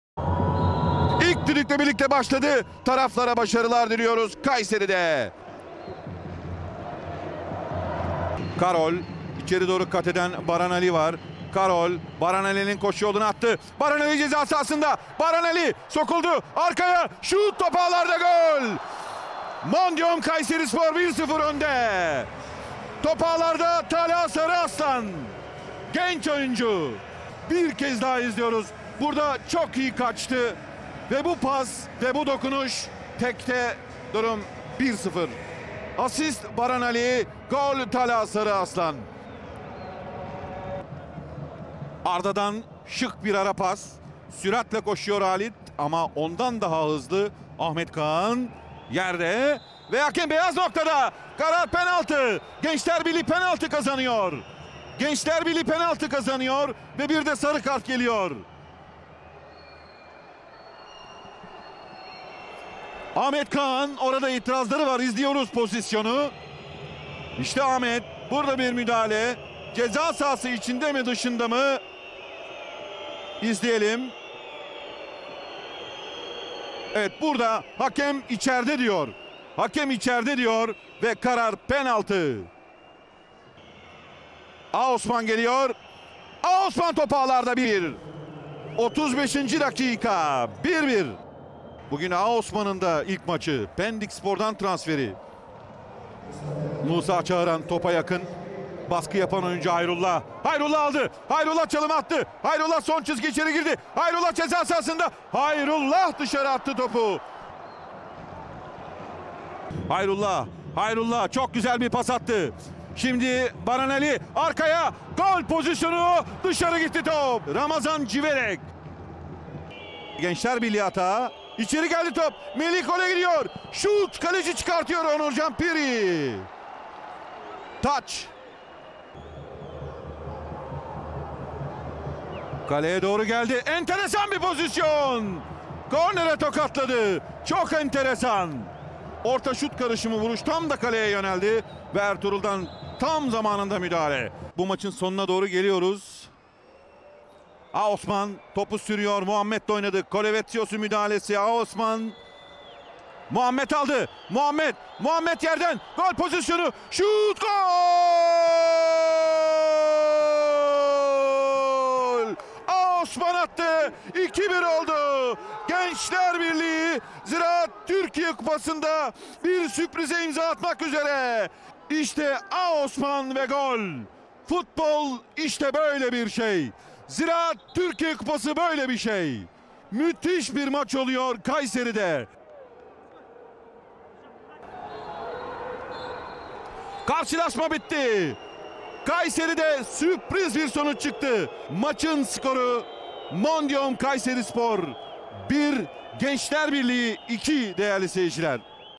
Kayserispor 1-2 Gençlerbirliği MAÇ ÖZETİ (Ziraat Türkiye Kupası 5. Tur) / 18.01.2024
Trendyol Süper Lig ekibi Mondihome Kayserispor ile TFF 1. Lig temsilcisi Gençlerbirliği, Ziraat Türkiye Kupası 5. tur maçında RHG Enertürk Enerji Stadyumu'nda kozlarını paylaştı.

Müsabakadan galip ayrılan ve adını son 16 turuna yazdıran taraf, 2-1'lik skorla Gençlerbirliği oldu.

Kırmızı-siyahlılara turu getiren golleri, 35. dakika (P) ve 90+4. dakikada Aosman kaydetti.

Ev sahibinin tek golü ise 23. dakikada Talha Sarıarslan'dan geldi.